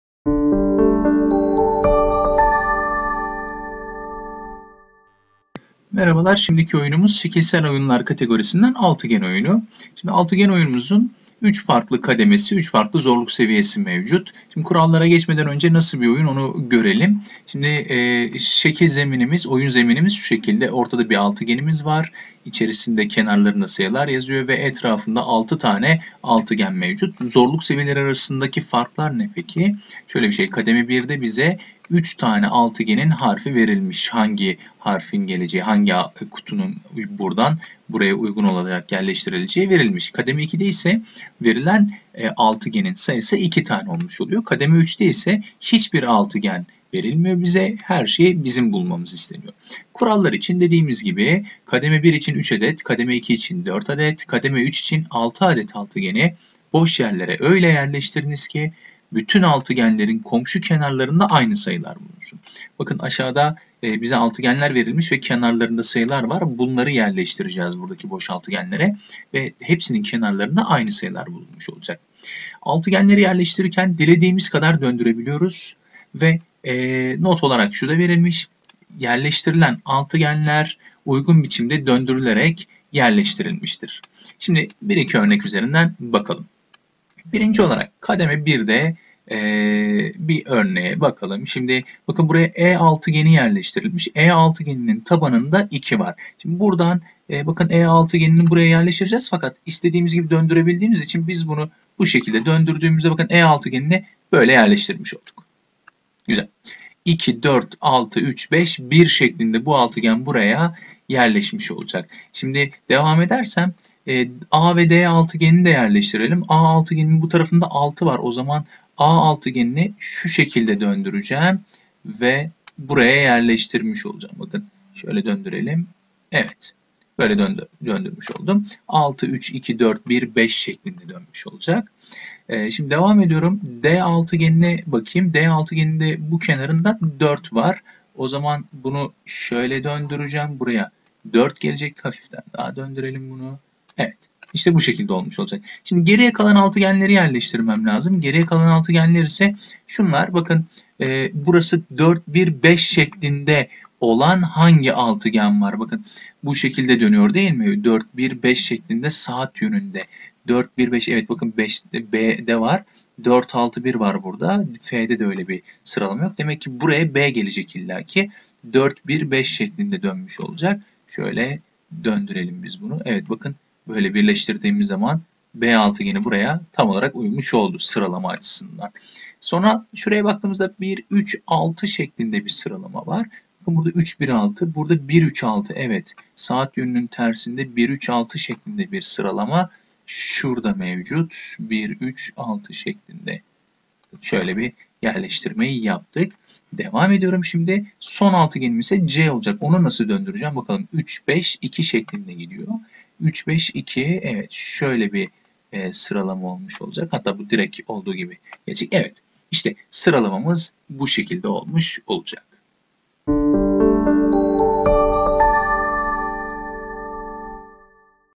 Akıl ve Zeka Oyunları -Şekilsel Oyunlar - Altıgen
Anlatım: Erdem Yamak-Şehit Ahmet Özsoy Fen lisesi
Konu anlatımında EBA-Mesleki Gelişim-Zeka Oyunları Eğitimi Çalışma Kitabı kaynak olarak kullanılmıştır.